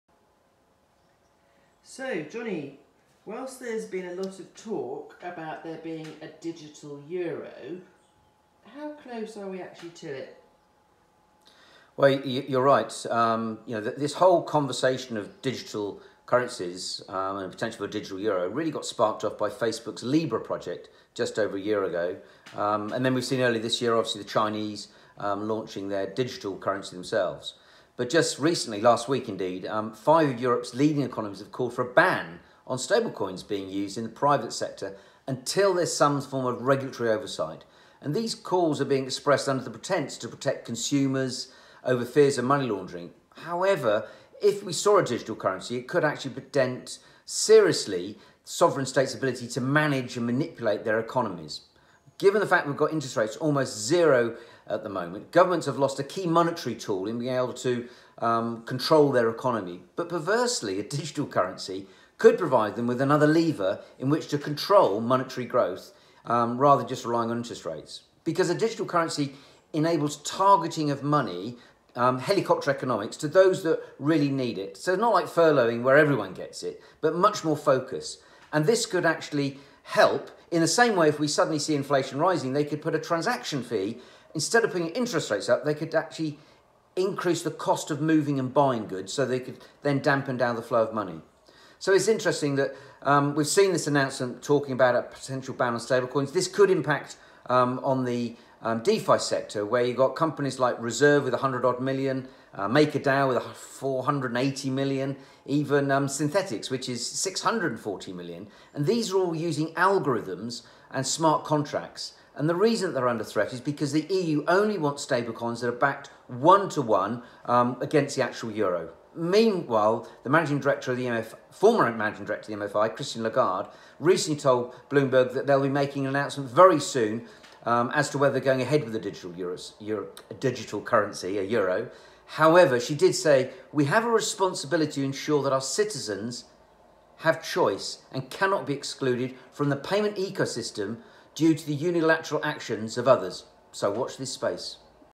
How close are we to a Digital Euro?
5 of Europe’s leading economies have called for a ban on stablecoins being used in the private sector until there is some form of regulatory oversight.
These calls are being expressed under the pretence to protect consumers and over fears of money laundering however it is a digital currency may well seriously dent sovereign states ability to manipulate and control their economies.
Given almost Zero interest rates governments have lost a key monetary tool perversely digital currencies could provide them with another lever to control monetary growth by imposing a transaction fee if inflation rises as a way to stop spending without raising interest rates. A Digital Currency also enables targeted giving of money – helicopter economics - not similar to furloughing but more focused on those that really need help
Could this recent announcement also pose a threat to the fast growing DeFi sector as it offers a number of ‘seigniorage-style’ coins such as $110 m Reserve  $479 million MakerDAO’s Dai, $640million  Synthetix which are managed using algorithms and smart contracts. This is because the EU only want stablecoins that are backed 1:1 with actual Euros
Meanwhile the former MD of the IMF now head of the ECB, Christine Lagarde, told  Bloomberg “it would soon be made public whether they will move forward with a eurozone-based digital currency.” Although she did add that "We have a responsibility to ensure that our citizens have choice and cannot be excluded from the payments ecosystem due to the unilateral actions of others,”